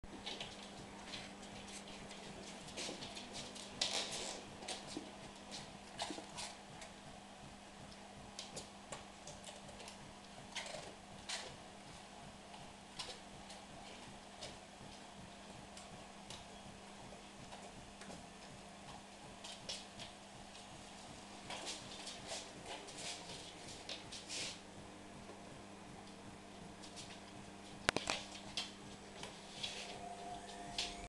Amy the hungry berne
Amy our sole surviving Berne abandons the breakfast table to get some toast.She is an old girl of nearly 12